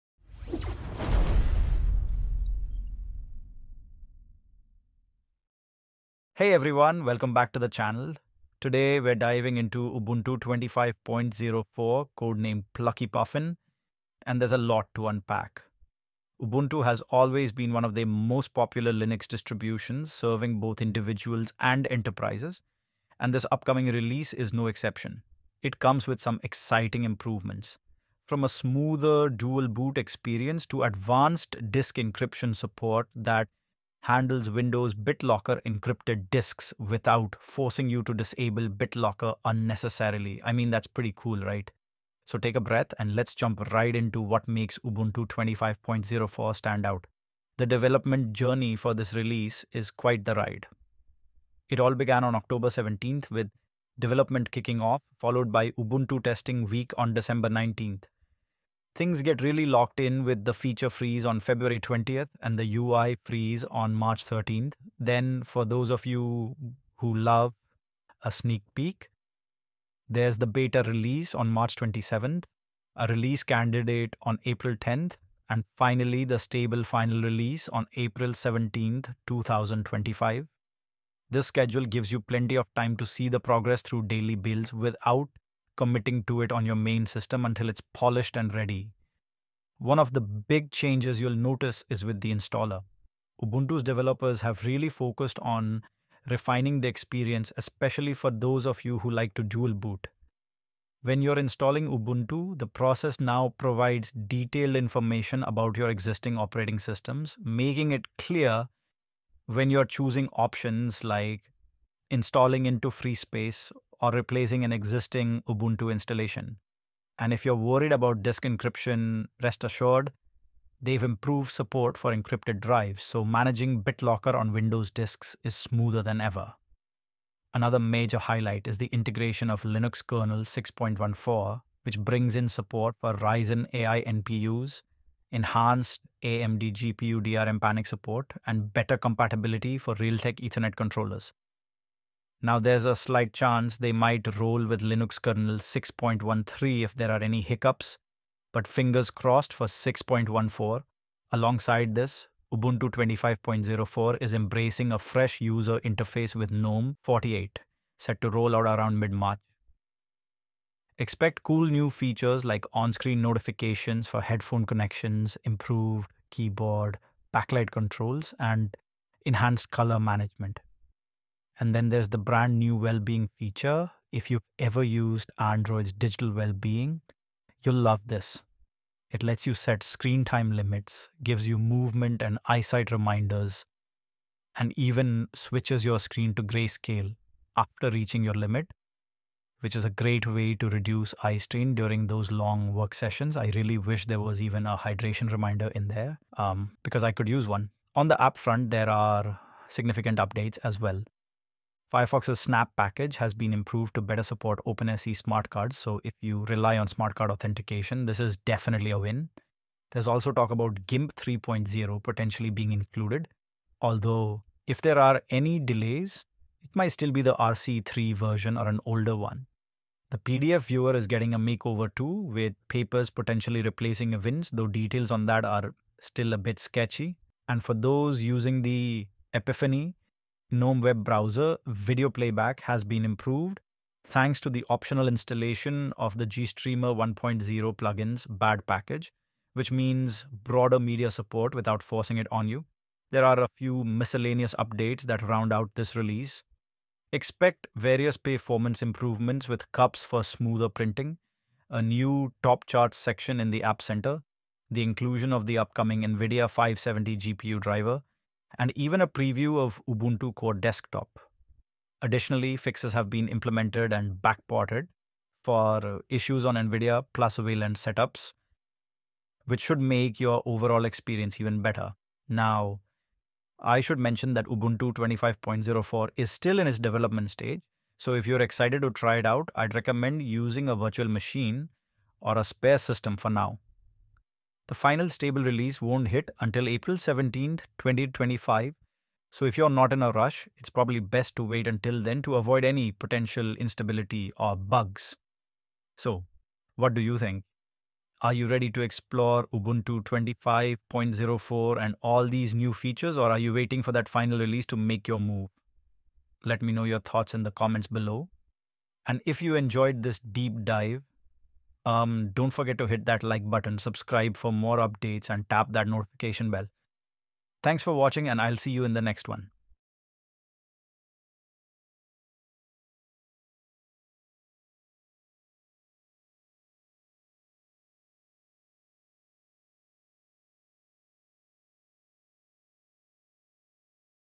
Ubuntu 25.04 "Plucky Puffin" Review: New Features, Linux Kernel 6.14 & Release Date Revealed!
Dive into our in-depth review of Ubuntu 25.04 "Plucky Puffin" and discover all the latest updates you need to know! In this video, we explore the revamped dual-boot experience, installer enhancements, and cutting-edge support for Linux Kernel 6.14, along with the fresh GNOME 48 upgrades. Learn how the new Wellbeing feature helps manage screen time and reduces eye strain, plus get insights on app updates including improvements to Firefox’s Snap package and potential changes to GIMP and the PDF viewer. With a detailed look at the complete release schedule leading up to the final launch on April 17, 2025, this video is your ultimate guide whether you're a seasoned Linux user or just starting out.